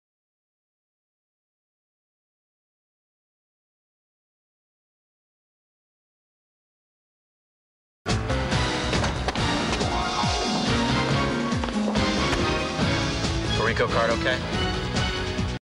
Leonardo DiCaprio in Japanese Credit Card Commercial 6
A young Leonardo DiCaprio in an old classic commercial